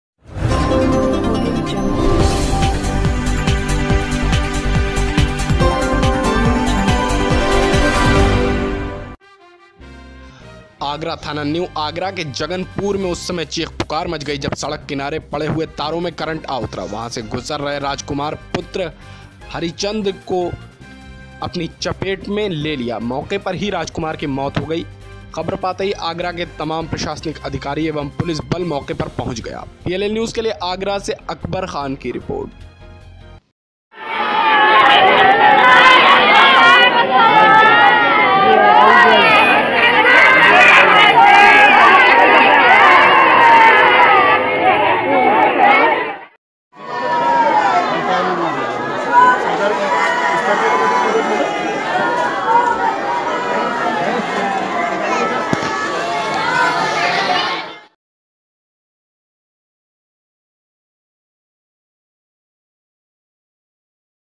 सड़क किनारे पड़े तारों में उतरा करंट, गुज़र रहे व्यक्ति की करंट लगने से मौत || AKBAR KHAN || PLL News
आगरा थाना न्यू आगरा के जगन पुर में उस समय चीख पुकार मच गई जब सड़क किनारे पड़े हुए तारों में करंट आ रहा था वहां से गुजर रहे  राजकुमार पुत्र हरिचंद को अपनी चपेट में ले लिया की मौके पर ही राजकुमार की  मौत हो गई खबर पाते ही आगरा के तमाम प्रशासन अधिकारी एवम पुलिस बल मौके पर पहुंच गया !

रिपोर्ट - अकबर खान